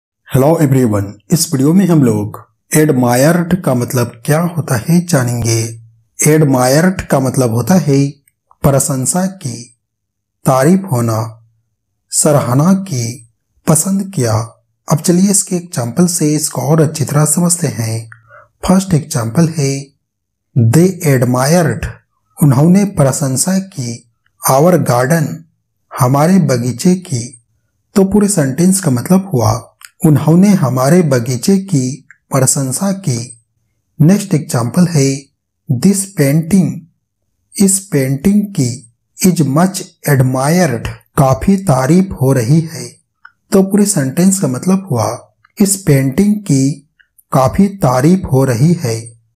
Admired Meaning in Hindi | Admired ka Matlab kya hota hai | Word Meaning English to Hindi
Admired Meaning in Hindi | Meaning of Admired | Admired का हिंदी में मतलब | English to Hindi vocabulary |

Aaj ki Is Video Me Maine Aapko Admired Ka Matlab Kya hota hai Hindi mein, Admired Ka definition or Admired Word Ke Sentence ka Example ko batlaya hai.